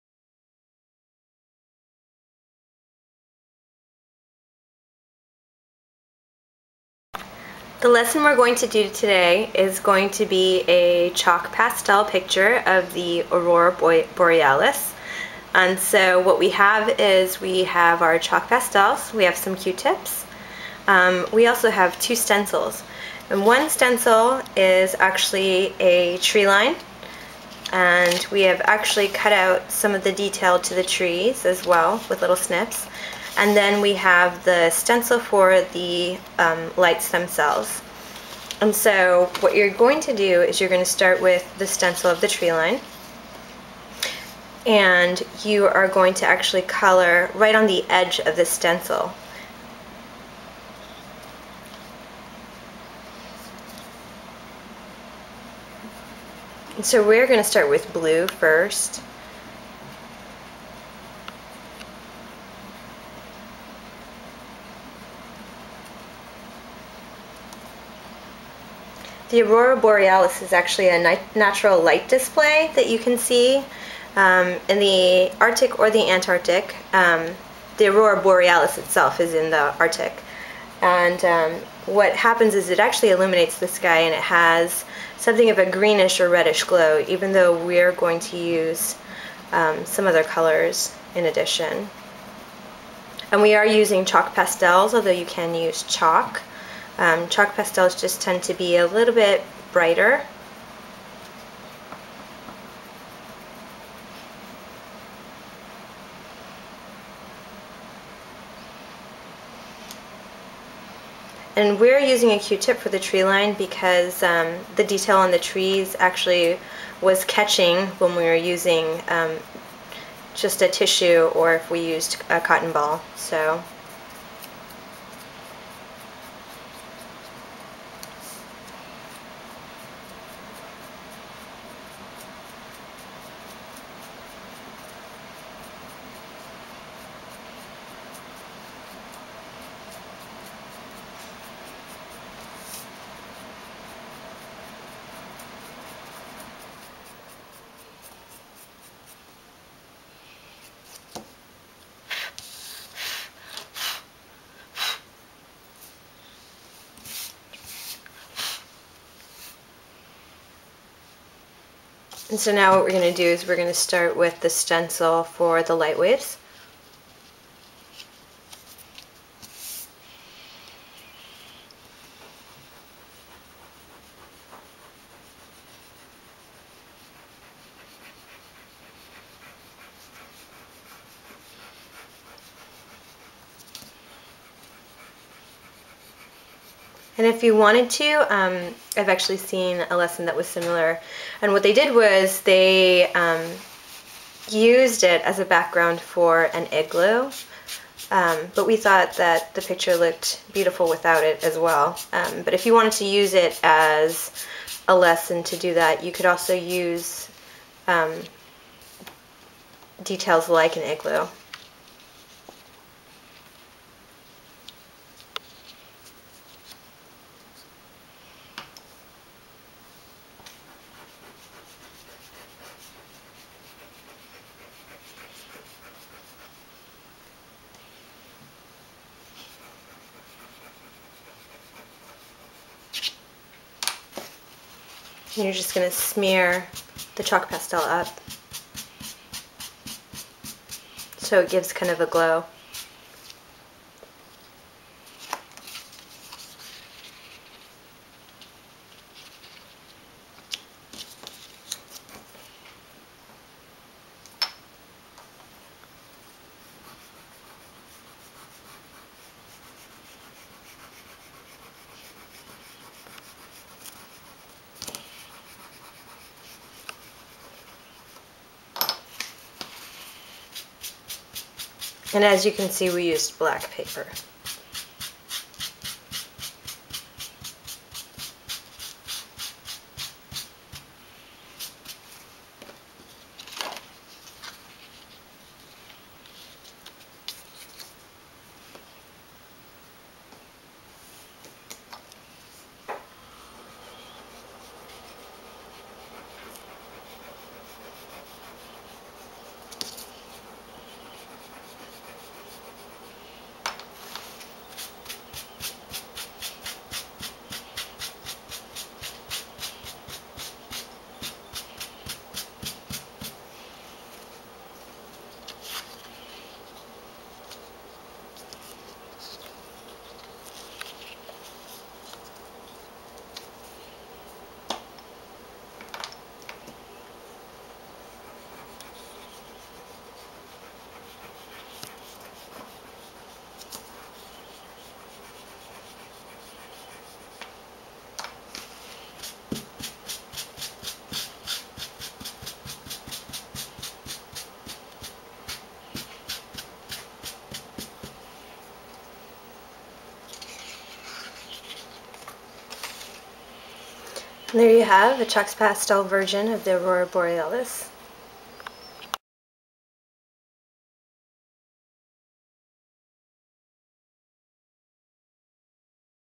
Northern Lights Preschool Chalk Pastel Lesson
The following art lesson creates a tree line and glowing lights with the use of stencils and chalk pastels.  Children preschool age to early elementary will enjoy completing this lesson to create the Northern Lights.

*If you are interested in more information about this lesson or interested in others by wecreate please visit us We your comments, questions, and subscriptions!